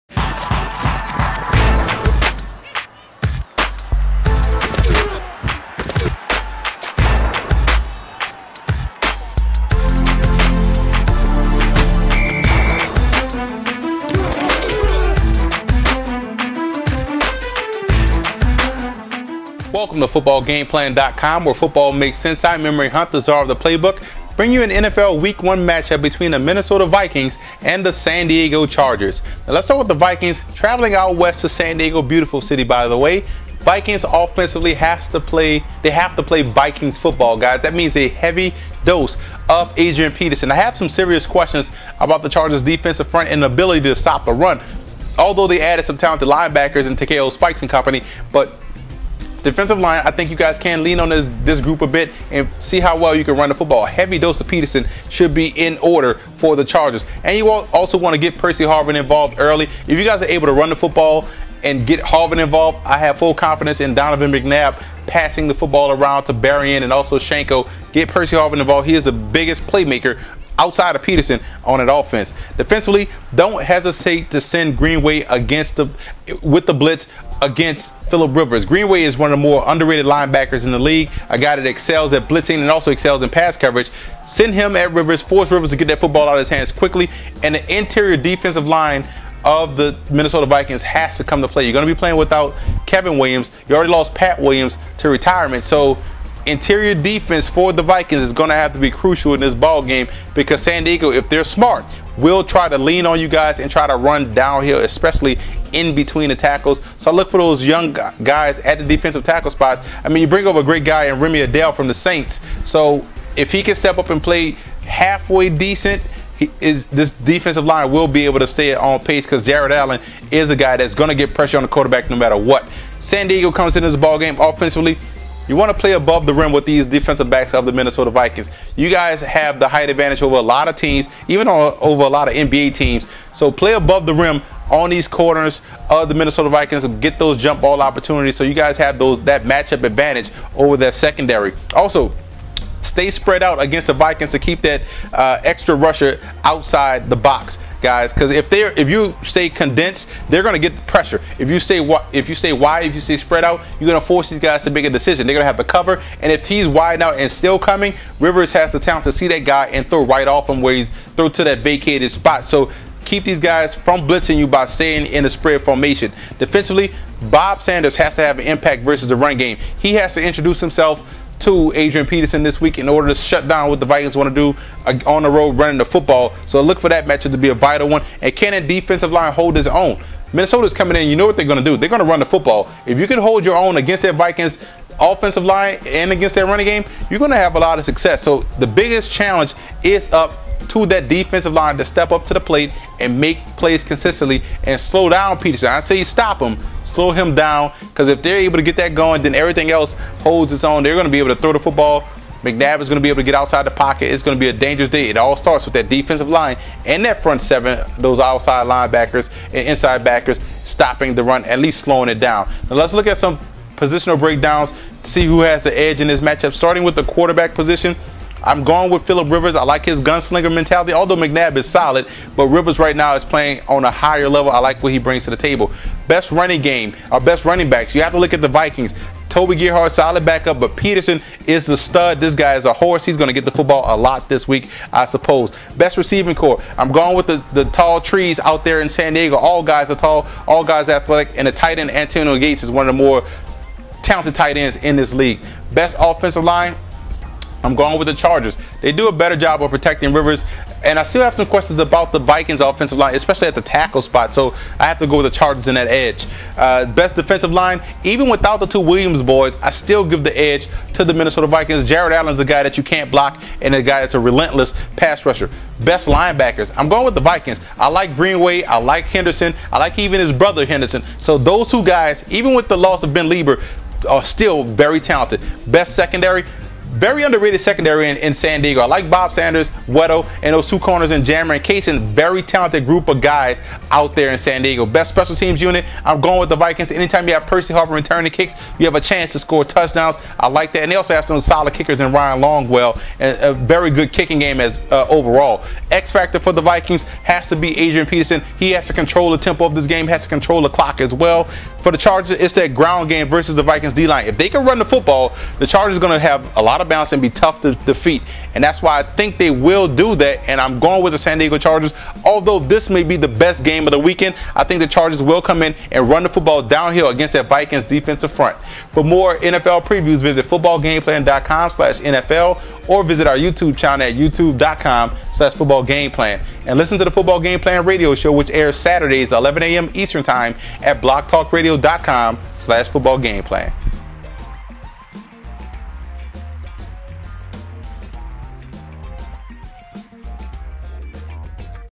Football Gameplan's NFL Preview Week 1 (Minnesota vs San Diego)
2011 NFL Week 1 Preview

Minnesota Vikings vs San Diego Chargers